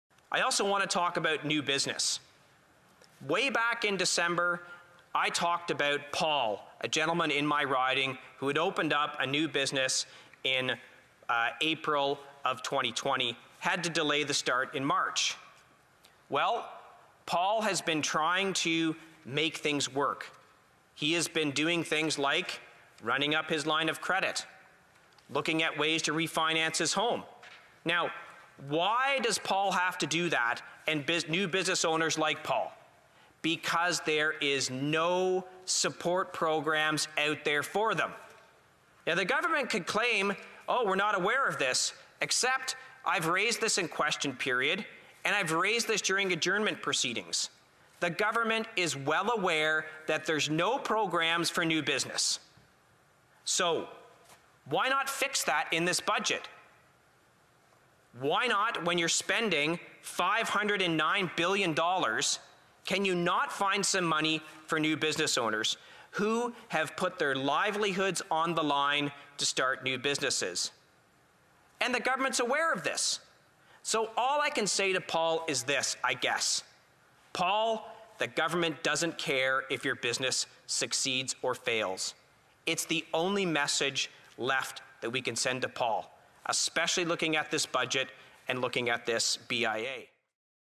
New Businesses Have No Support from This Government
I've been fighting for support for new businesses for a year. The Liberal government gives them nothing.  Instead, the government let large corporations pad their bottom lines with the wage subsidy.